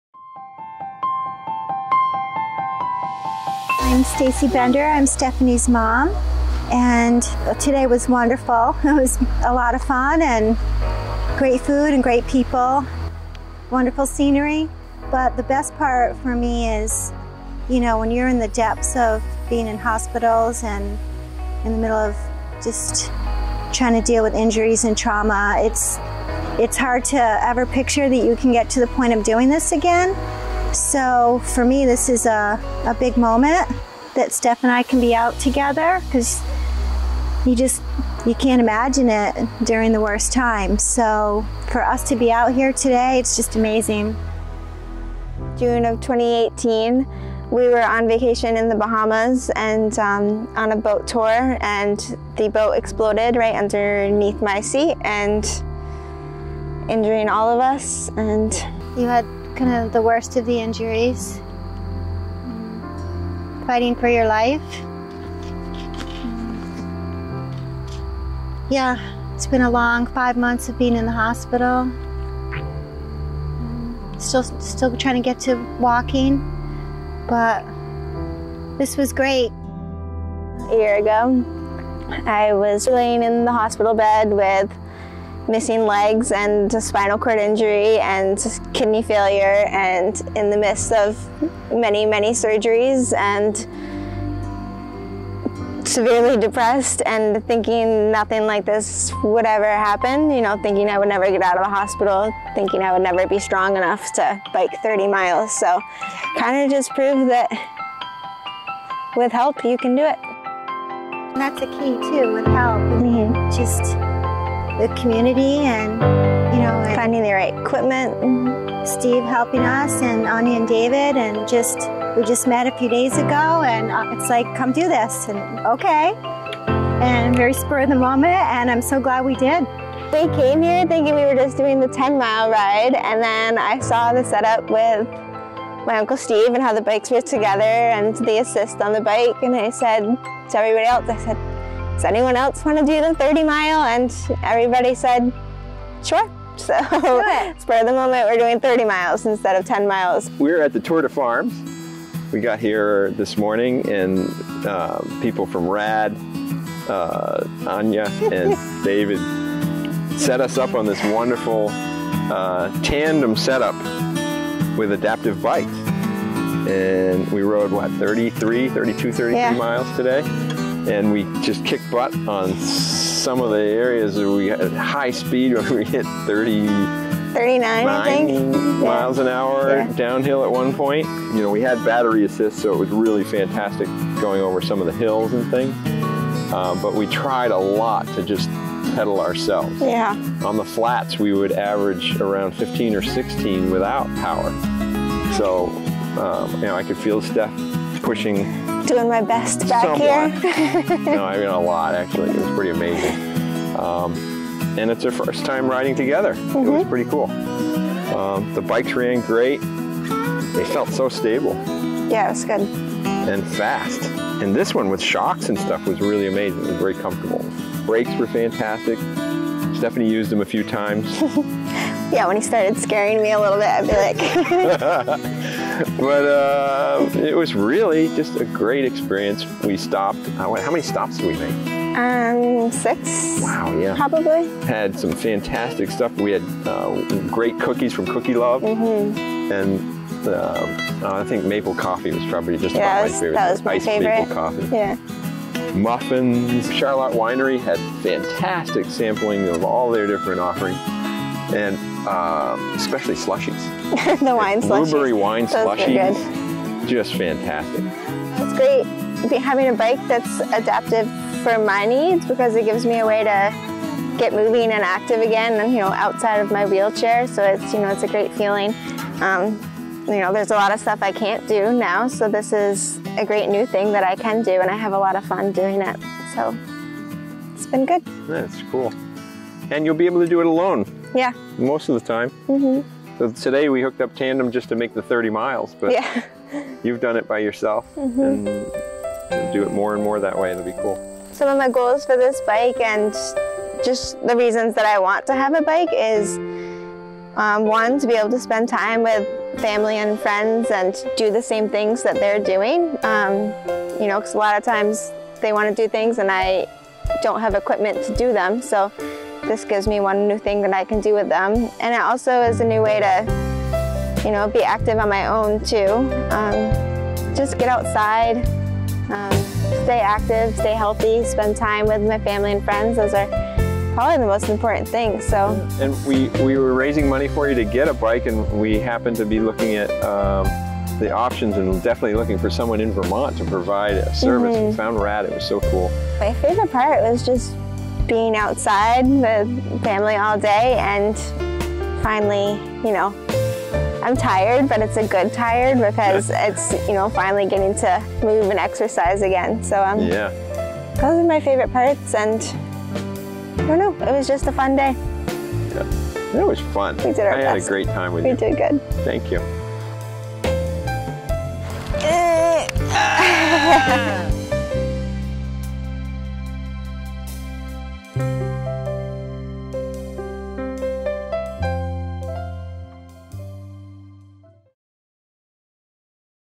Hase Bikes Kettwiesels in tandem: adaptive & inclusive cycling!
Stefanie Schaffer and her family joined us at the Tour de Farms to spend some time on some  HASE Bikes recumbent trikes. Stefanie's story of recovery is an inspiration and we're honored to be able to help her move forward on her journey toward freedom and mobility.